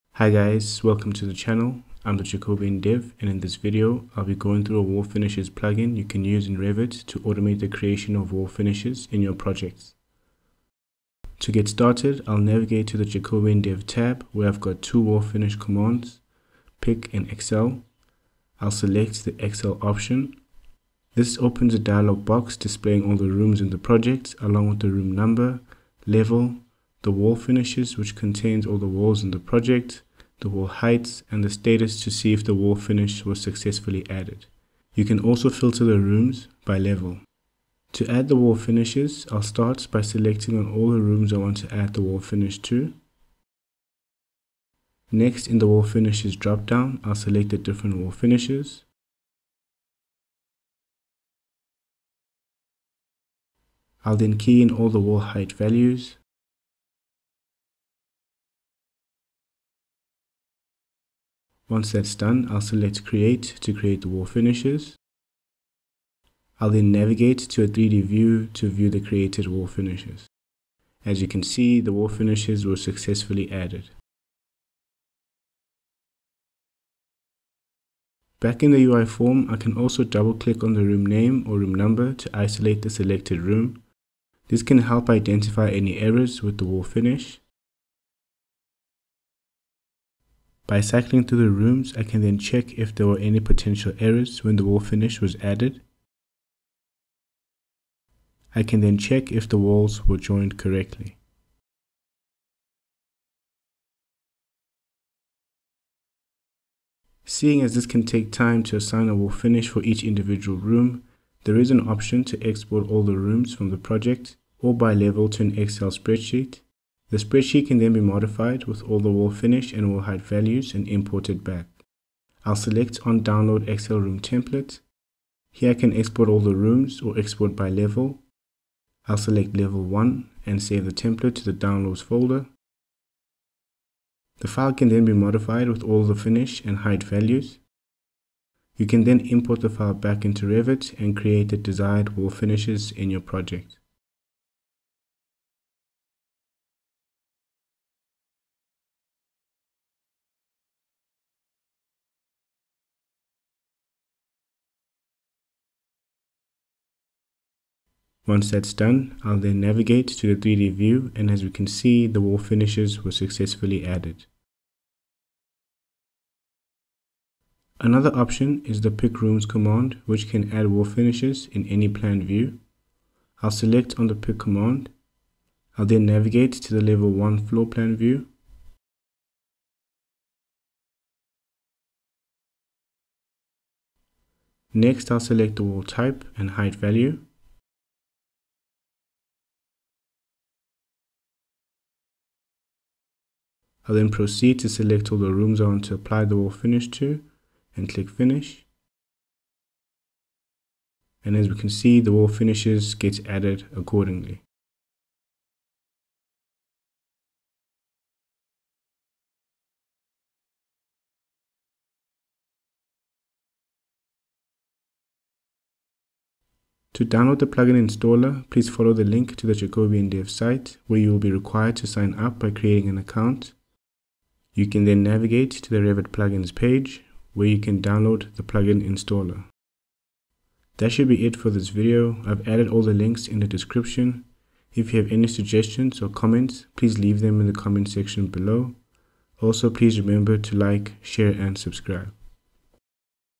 Automated Wall Finishes In Revit
The video goes through a Revit Plugin that automates the creation of wall finishes in Revit.

Link to Jacobian Dev Site